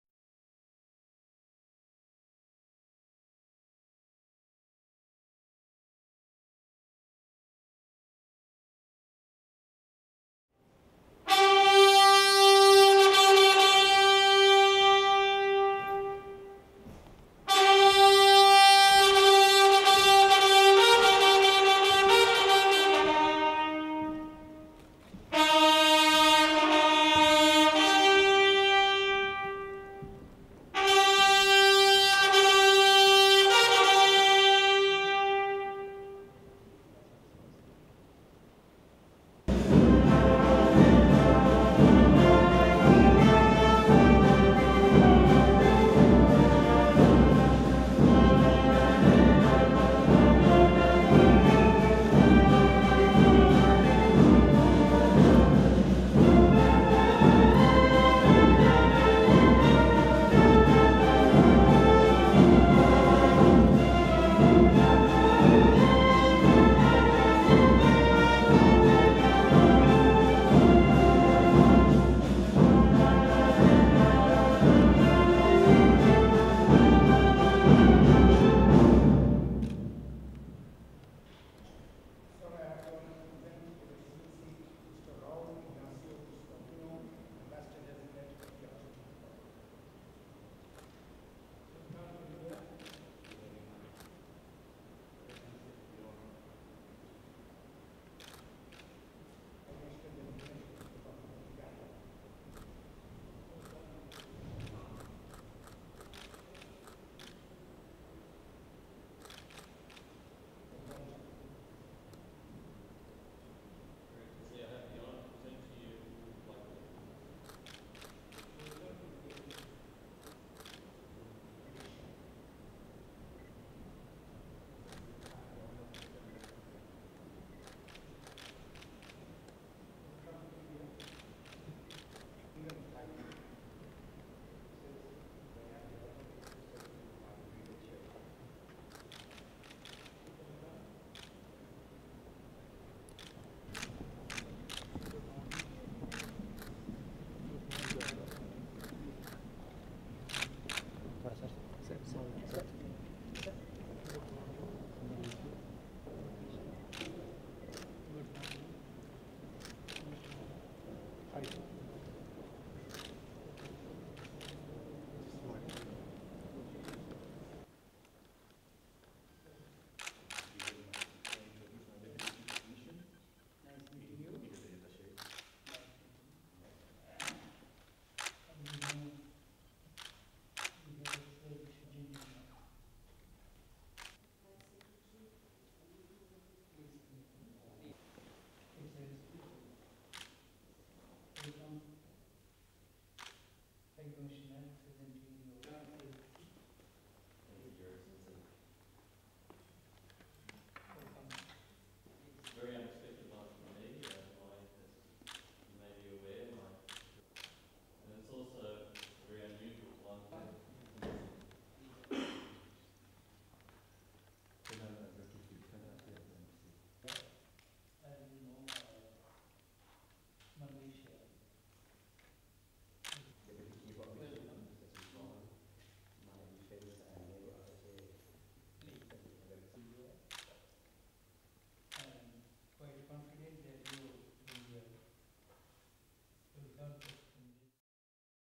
Envoys of Five Nations present their Credentials to President Mukherjee
Envoys of Argentina, Uganda, Malta, Malaysia and Thailand presented their credentials to the President of India, Shri Pranab Mukherjee at a ceremony held in Rashtrapati Bhavan today (December 16, 2013).

The envoys who presented their credentials were: -

1. His Excellency Mr. Raul Ignacio Guastavino, Ambassador of the Argentine Republic.

2. Her Excellency Ms. Elizabeth Paula Napeyok, High Commissioner of the Republic of Uganda

3. His Excellency Mr. John Joseph Aquilina, High Commissioner of the Republic of Malta

4. His Excellency Datuk Naimun Ashakli Bin Mohammad, High Commissioner of the Malaysia

5. His Excellency Mr. Chalit Manityakul, Ambassador of the Kingdom of Thailand